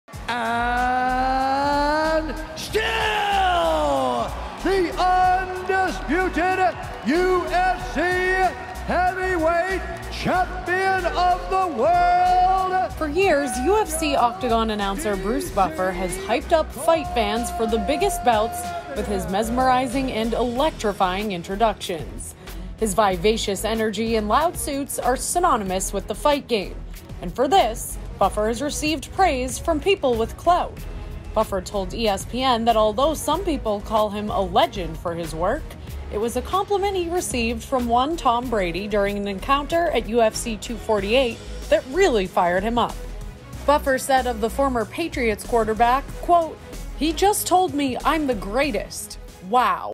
Bruce Buffer On Getting Compliment From Tom Brady
UFC octagon announcer Bruce Buffer talked about a compliment he received from former New England Patriots quarterback Tom Brady.